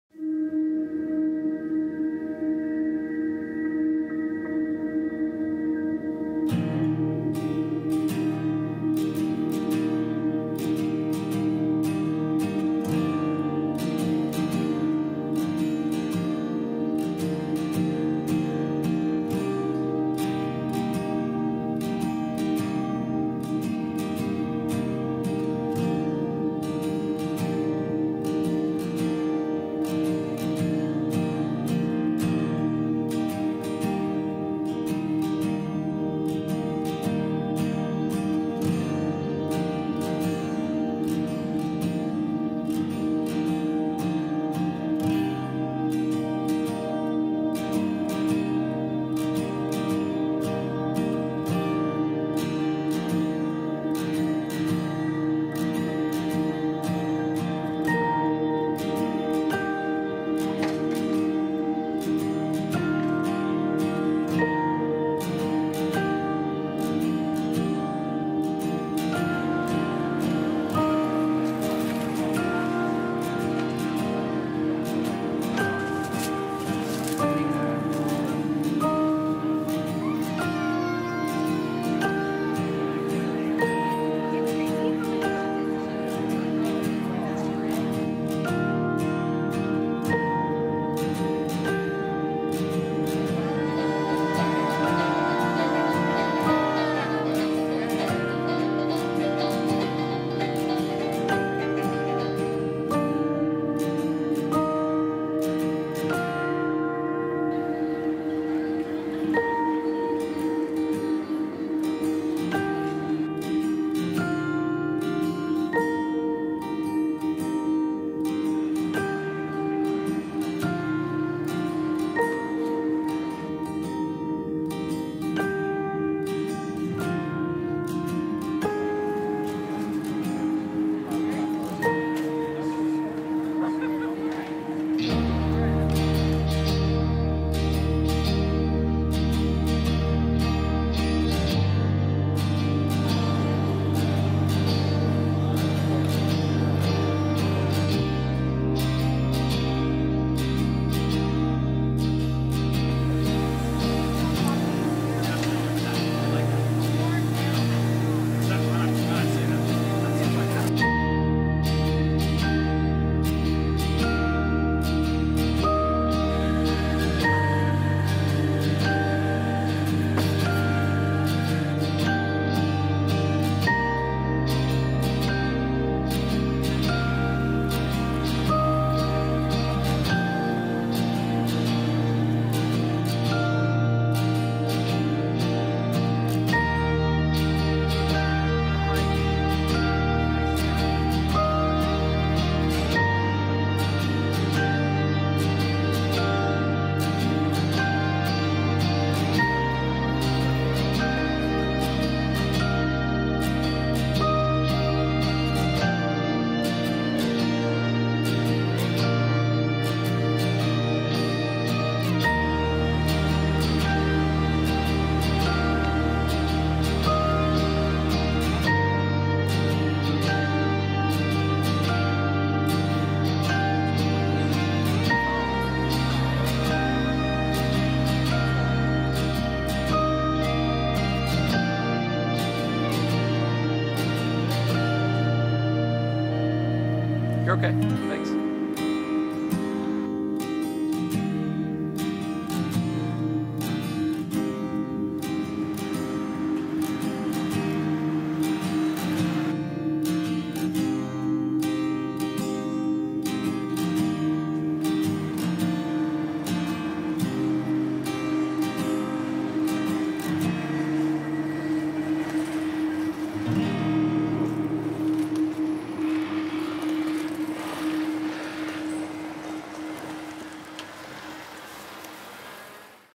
Kentmere 400 at the Portland Farmer's Market
I took a roll of Kentmere 400 Black and White Negative film out to the Portland Farmer's Market and Portland State University a few weeks ago to test out what it looks like and get some street shots. I'm very pleased with Kentmere 400 to be honest. The grain is subtle, the whites and blacks are more grey which feels a little more balanced to me. More detail in the shadows is never a bad thing. Which ones were your favorites? Thanks as always for stopping by, friends.